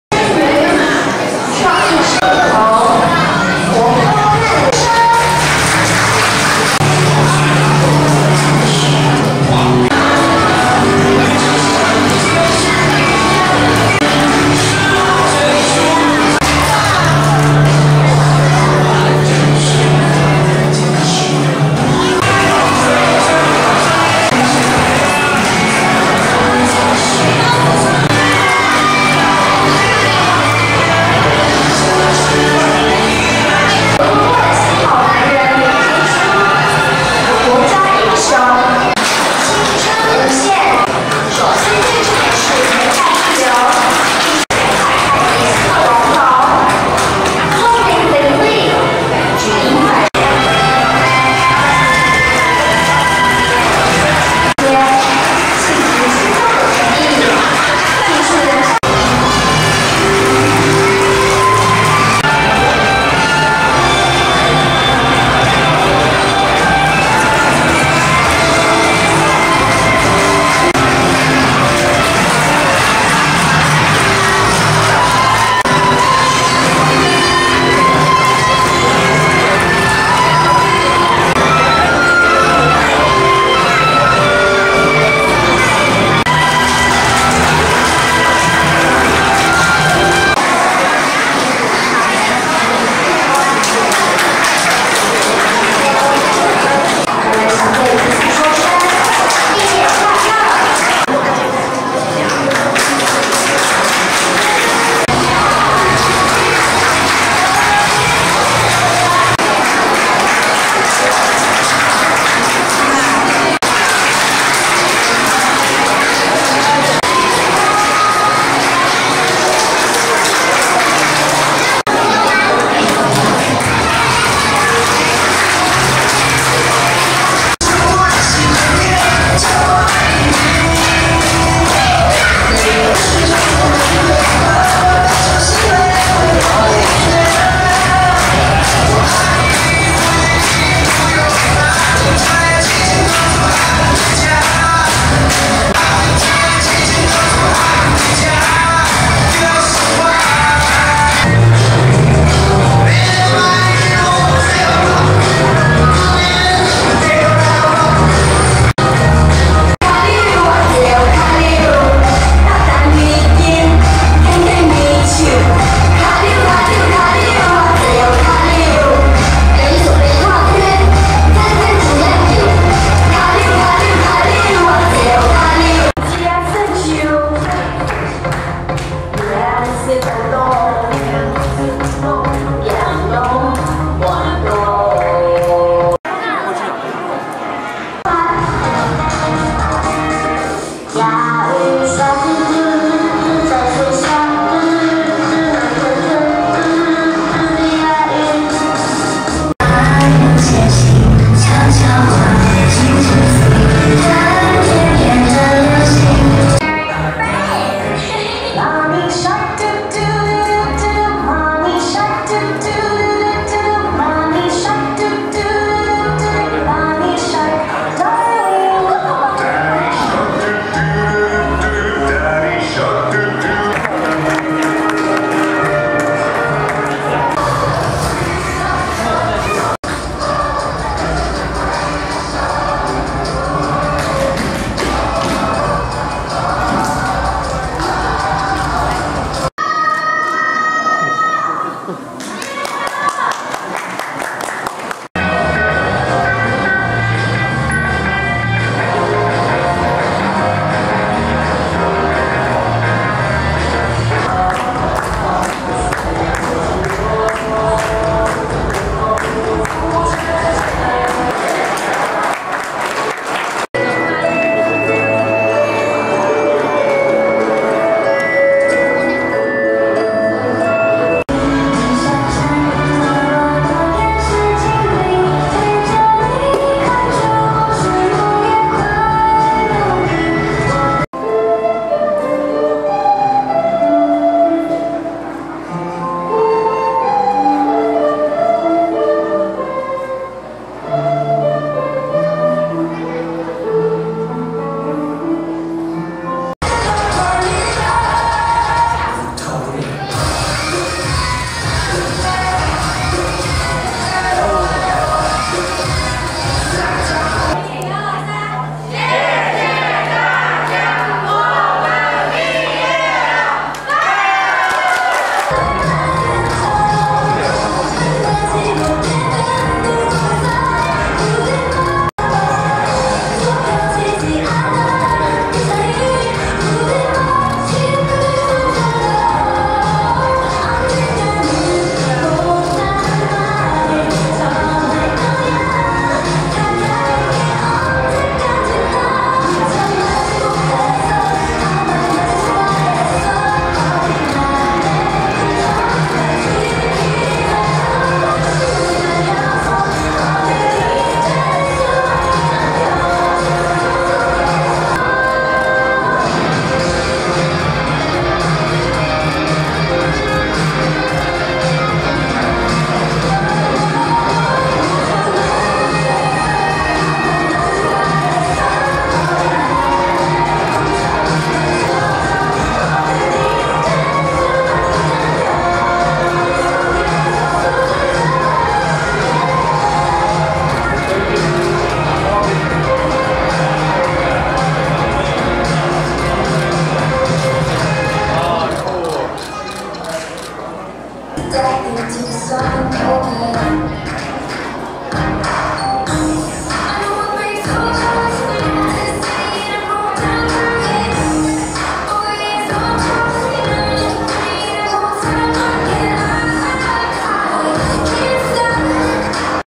馬祖日報2018/06/20影音／很搖滾！ 東引國中小19日畢典晚會舞動青春
日期：2018/06/19晚間19:00
地點：東引國中小禮堂
攝影：陳其敏
　搖滾樂力量已超出了一種音樂形式，它影響了一代人生活方式、時尚態度，深受年輕人喜愛。畢業典禮留下不一樣記憶，東引國中小106學年畢典晚會主題「盛夏搖滾、舞動青春」，充分掌握年輕、活力、青春節奏，也給青少年體悟青春無價。
　在眾目的聚焦下，幼兒園、國小、國中畢業生在家長陪同下，亮麗進場，接受眾人的喝采，也交織家庭幸福滿分畫面，在愛的環境下成長，子女正向能量，更具信心接受挑戰。